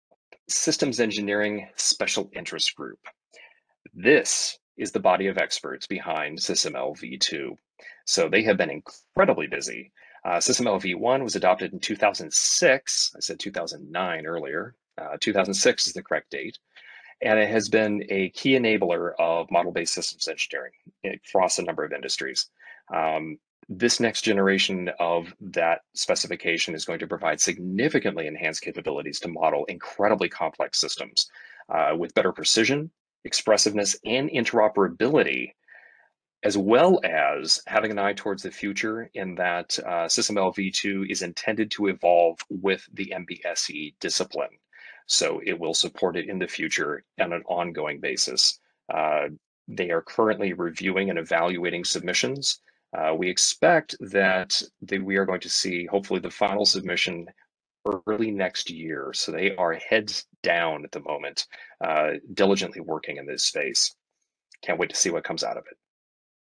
Systems Engineering Domain Special Interest Group (SE DSIG) Update
The SE DSIG provides a standard systems modeling language to specify, design, and verify complex systems, facilitates integration of systems and software engineering disciplines, and promotes rigor in the transfer of information between disciplines and tools for developing systems.